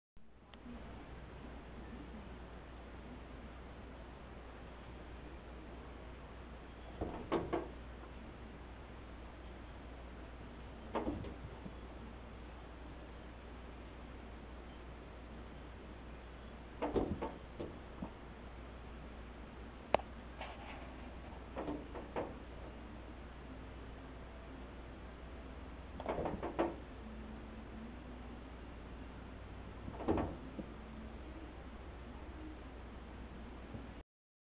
2010 May 6 Chickadee.avi
This chickadee has been flying against the windows in the house for several days now.  Over and over and over and over.  Driving the cats crazy and I'm not far behind!